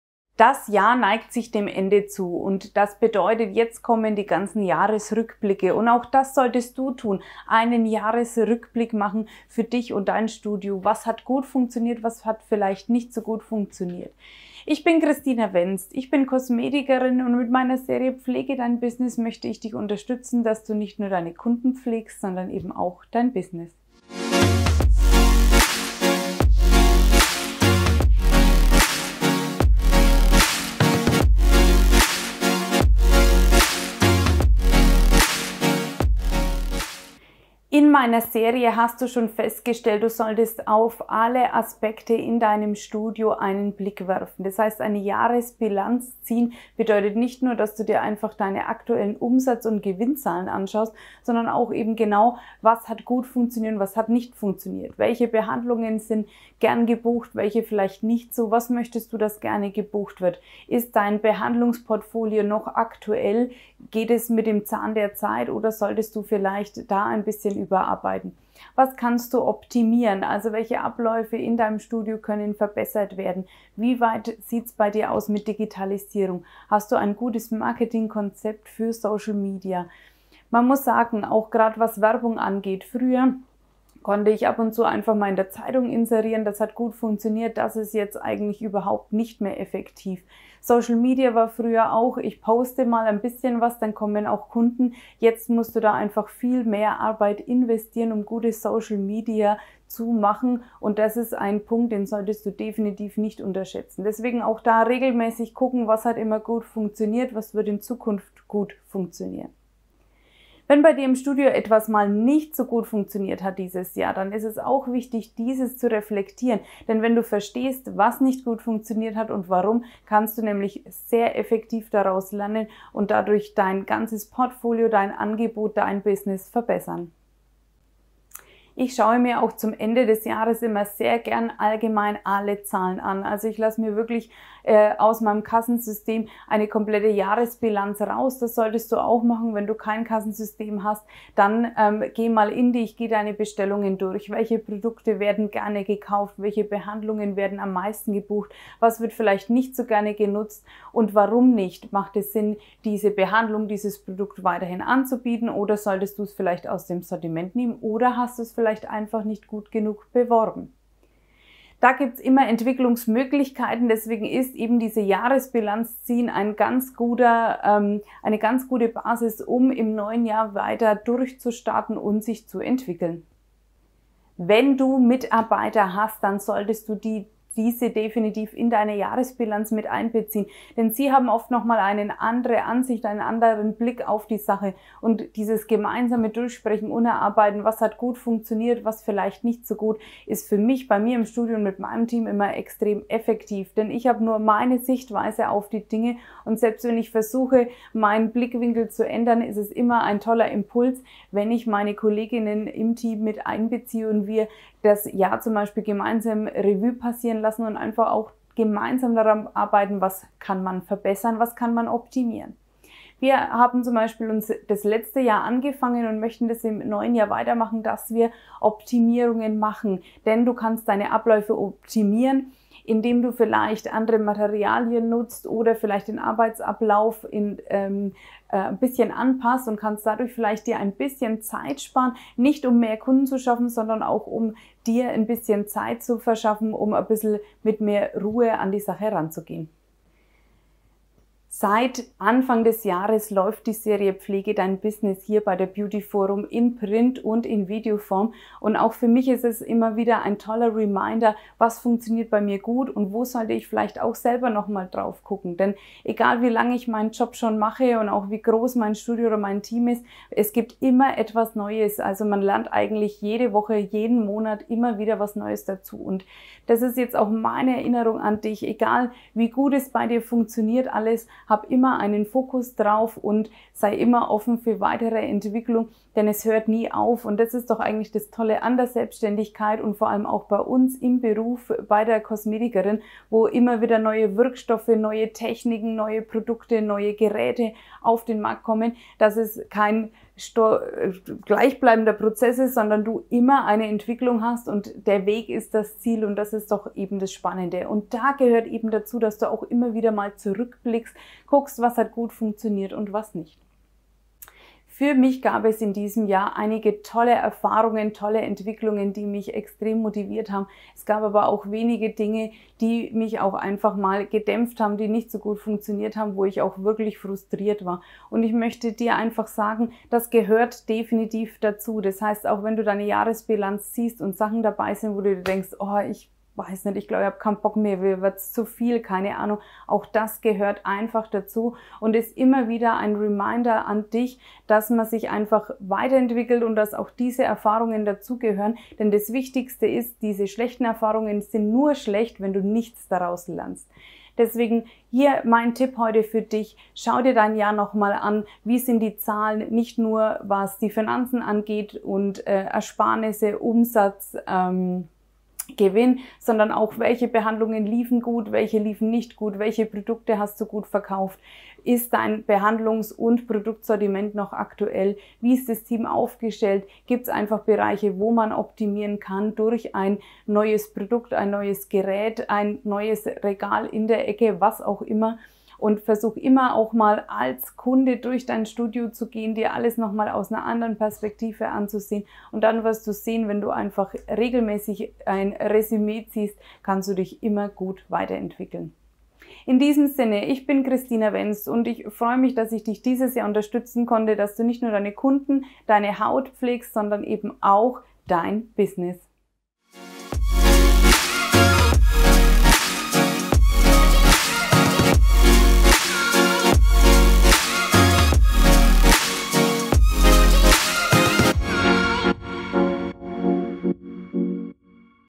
Teil 12: Zeit für die Jahresbilanz – was lief gut und was gibt es zu verbessern?
Pflegen, pflegen, pflegen: Das ist das Credo für die Haut der Kundschaft und auch für das Business! Kosmetikerin und Beauty-Coach Christina Wenst gibt Tipps, wie das Kosmetikinstitut in Schuss bleibt. In Teil 12 ist es Zeit für die Jahresbilanz. Als Unternehmerin sollte nun zurückgeblickt und analysiert werden: Was lief gut und was gilt es zu ändern?